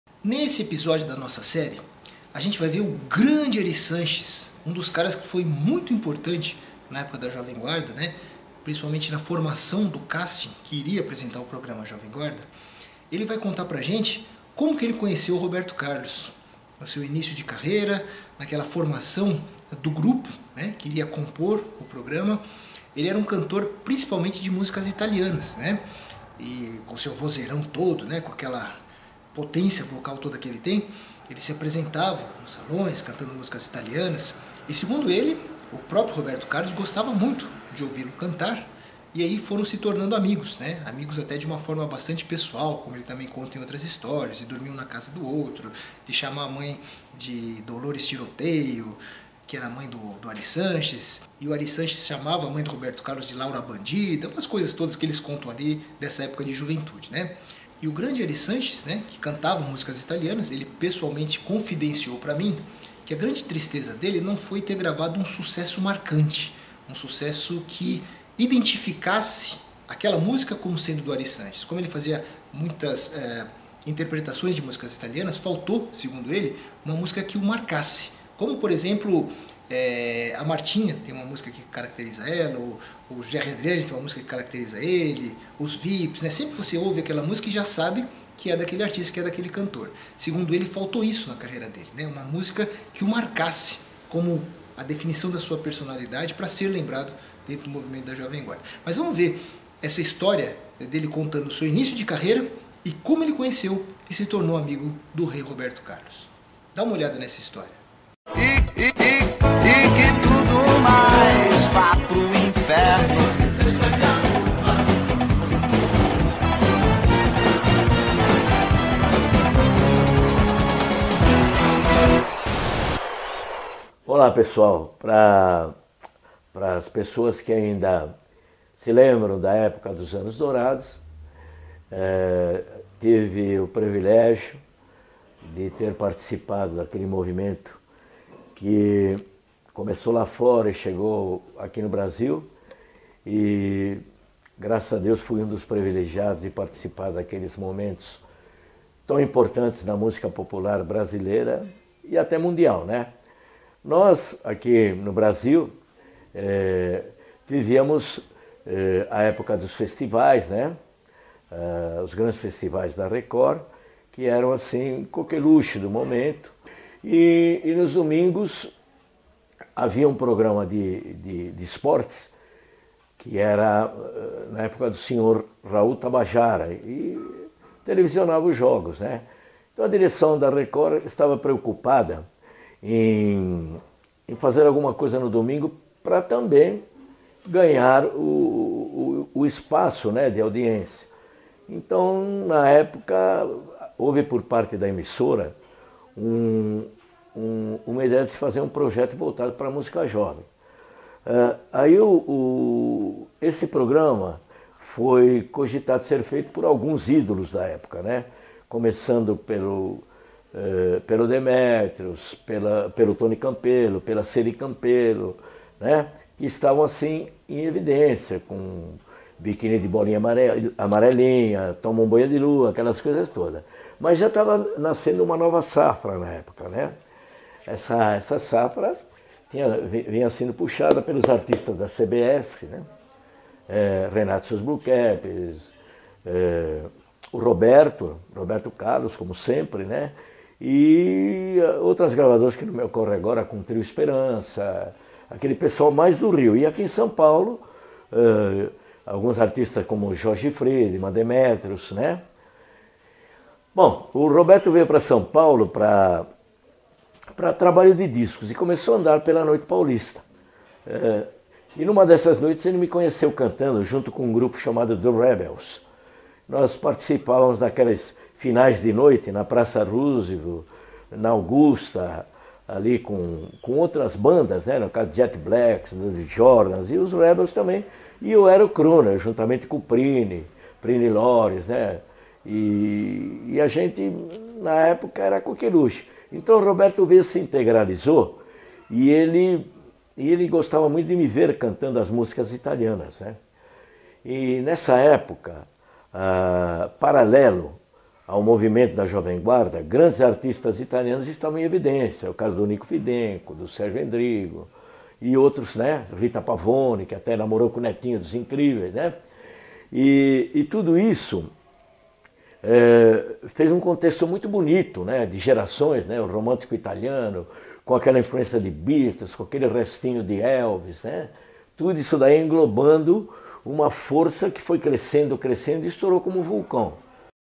COMO CONHECI ROBERTO CARLOS (Episódio 81)
Neste episódio o cantor Ary Sanches conta sobre o início de sua carreira e como Roberto Carlos o admirava cantando músicas italianas na noite paulistana.